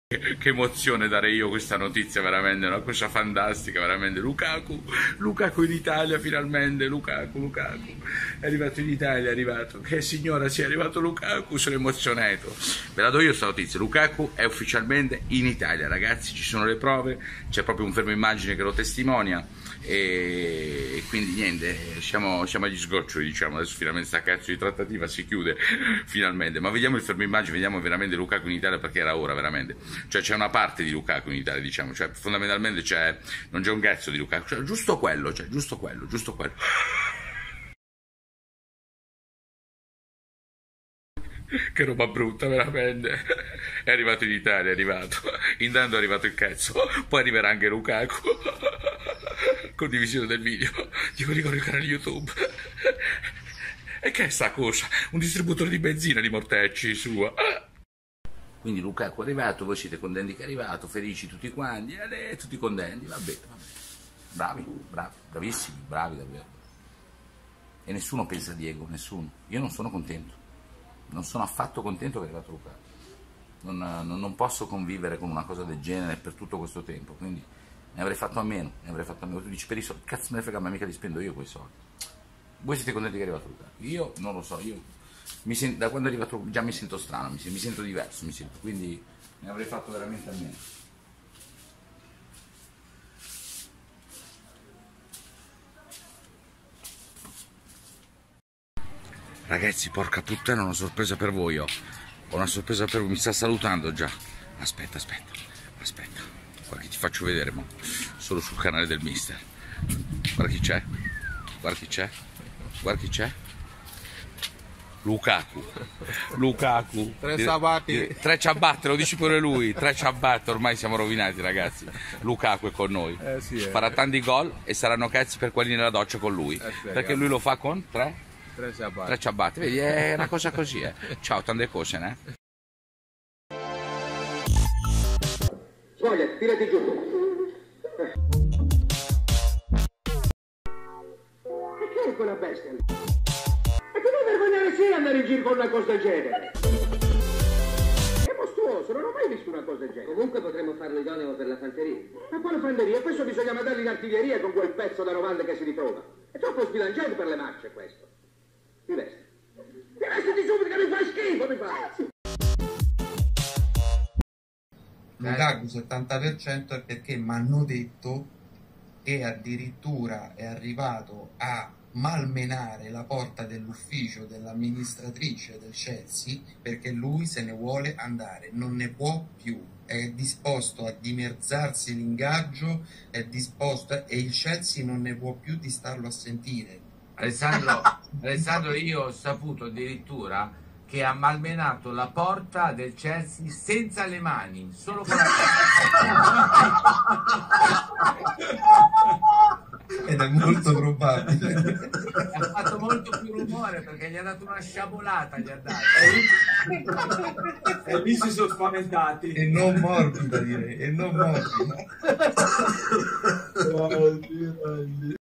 QUELLO CHE NON AVETE MAI SAPUTO SU LUKAKU! DAL SUO ARRIVO-VISITE MEDICHE E I CASINI AL CHELSEA ORA!!
ISCRIZIONE CANALE FULVIO COLLOVATI OFFICIAL PER I MIEI INTERVENTI A CALCISSIMO - TOP CALCIO 24 :